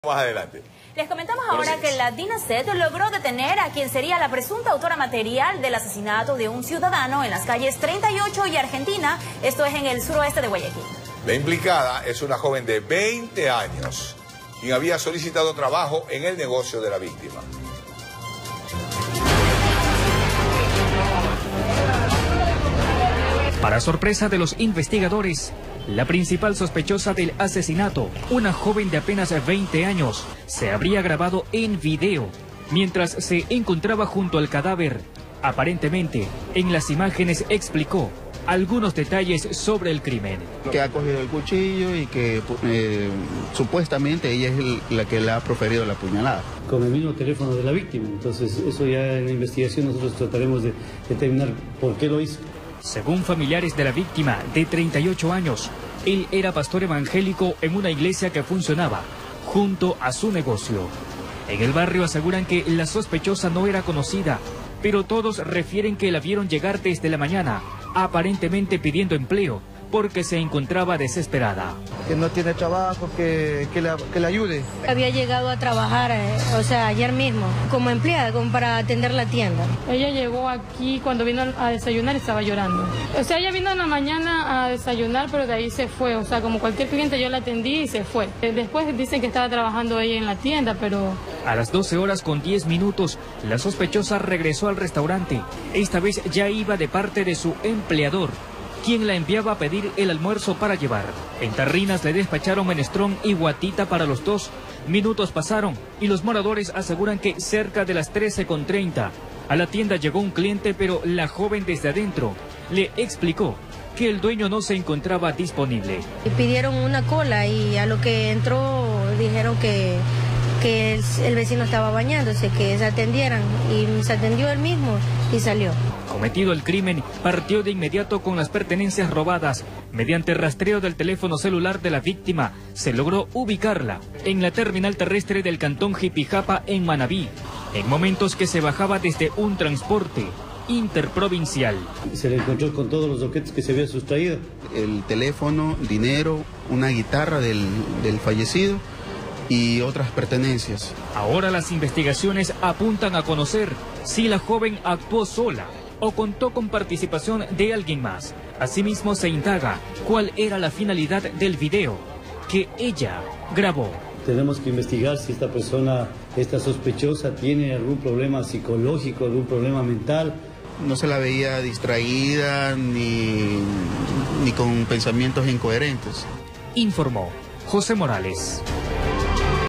Detienen a sospechosa de asesinato de Pastor Evangélico
La presunta agresora se habría grabado en video

El crimen se registró en el suroeste de Guayaquil. La joven señalada tiene apenas 20 años de edad.